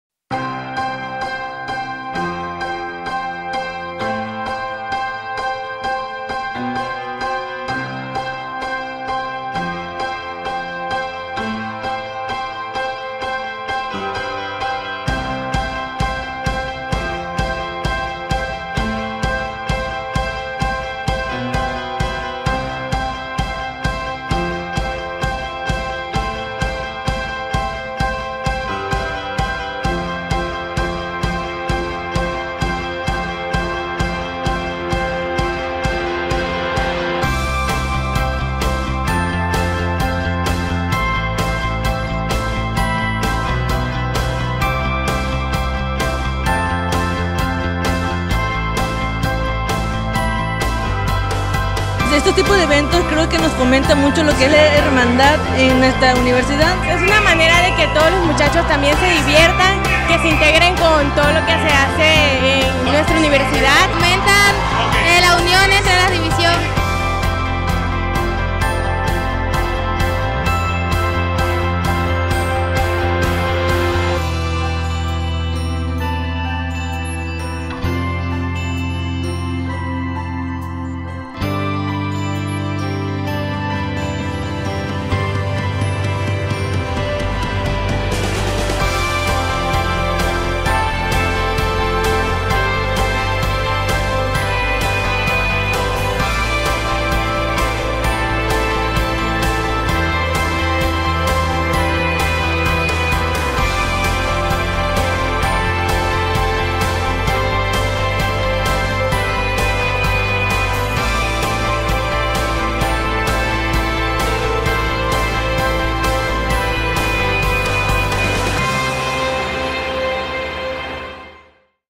Caravana Universitaria DAMC
Gracias a la comunidad de DAMC que participó activamente en esta segunda Caravana. Pronto visitaremos a las otras divisiones.